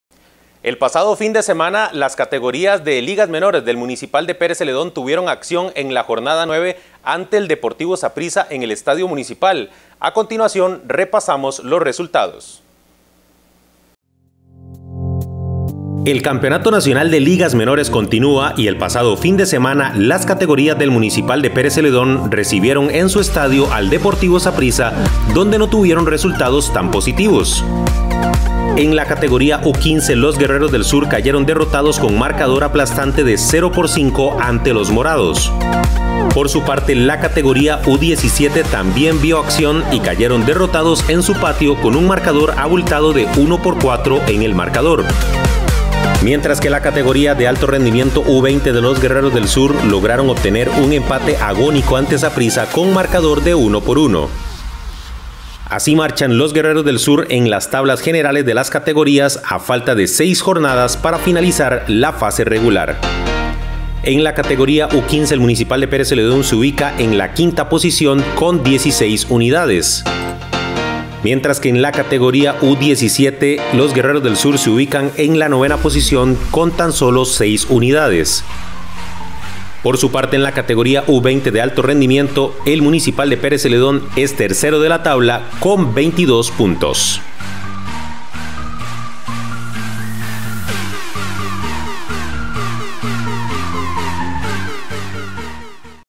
Fútbol: Ligas Menores del Municipal PZ recibieron a Saprissa por la jornada 9 de torneo
El campeonato nacional de Ligas Menores continúa y el pasado fin de semana las categorías del Municipal de Pérez Zeledón recibieron en su estadio al Deportivo Saprissa donde no tuvieron resultados tan positivos.
En la categoría U-15, los Guerreros del Sur cayeron derrotados con marcador aplastante de 0-5 ante los morados.
Por su parte, la categoría U-17 también vio acción y cayeron derrotados en su patio con un marcador abultado de 1-4 en el marcador.
Mientras que la categoría de alto rendimiento U-20 de los Guerreros del Sur lograron obtener un empate agónico ante Saprissa con marcador de 1 por 1.
Así marchan los Guerreros del Sur en las tablas generales de las categorías U20 U17 y U15 a falta de seis jornadas para finalizar la fase regular.

Nota por: Periodista Daniel Araya
Canal: Cabletica TV - CTV Noticias